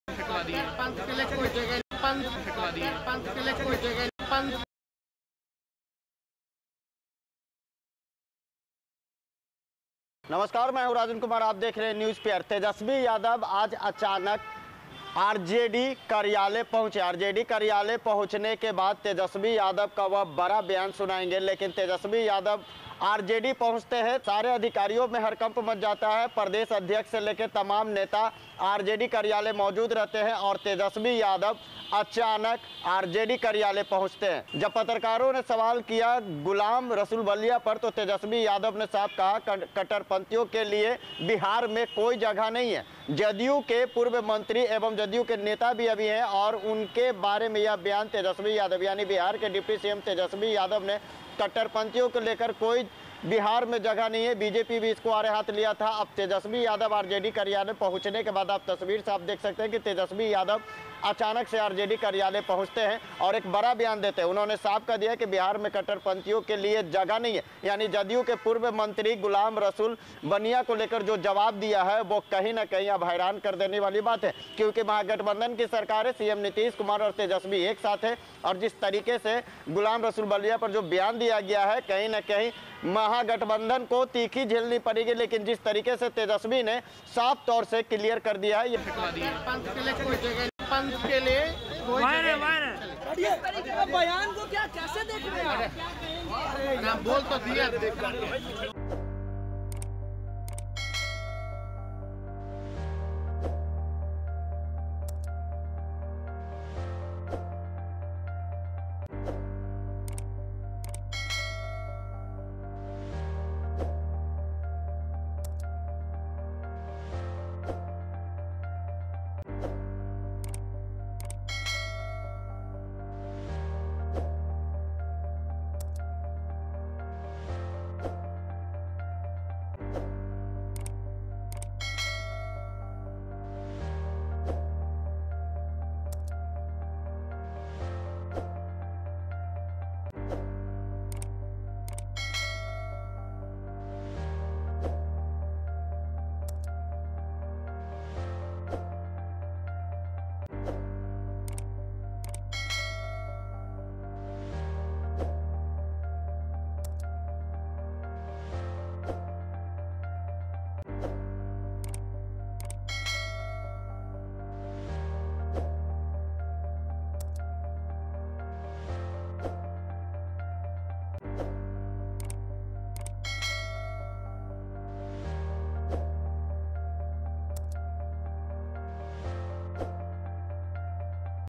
Tejashwi Yadav का JDU पर हमला! कहा कट्टरपंथ के लिए जगह नहीं ! देखिए किसपर है निशाना ?
धन्यवाद
टीम NewsPR